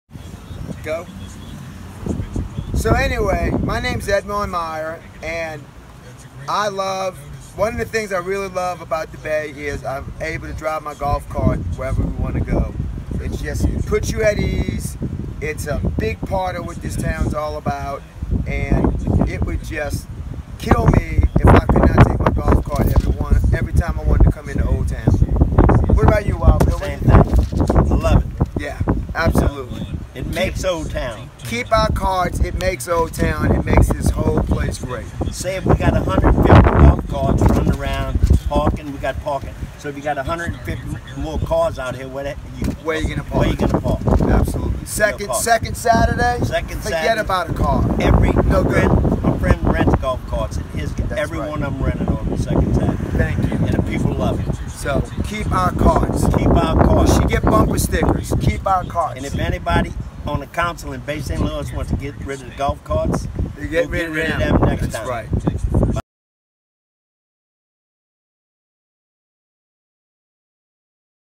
Golf cart fans say they're a way of life
Edmond Meyer and Wild Bill tell why golf carts are a way of life in Old Town Bay St. Louis.